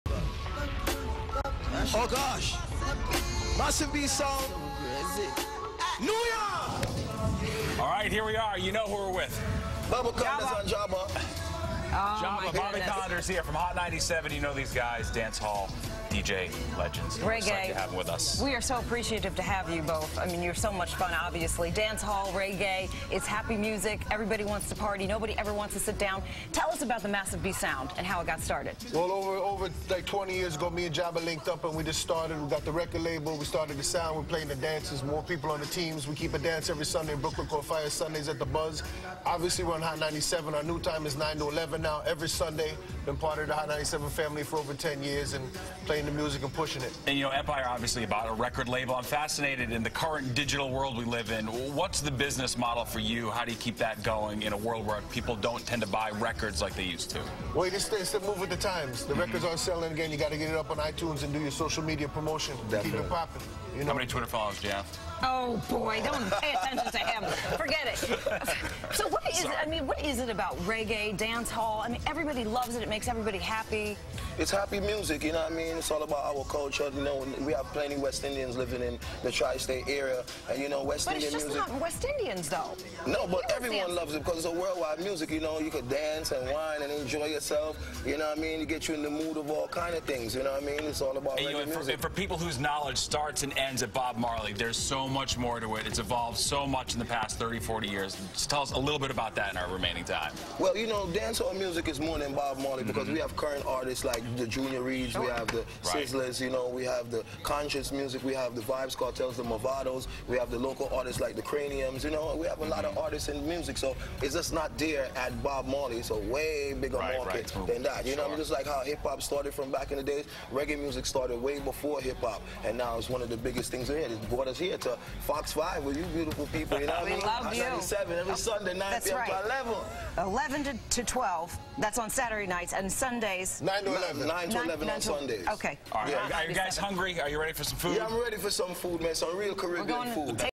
After Empire: DJs Bobby Konders and Jabba
New York Dancehall DJs Bobby Konders and Jabba were at the Fox 5 studio Wednesday night. They founded Massive B record label 10 years ago. Hear them on Hot 97 every Saturday from 11 p.m. to midnight and every Sunday from 9 p.m. to midnight.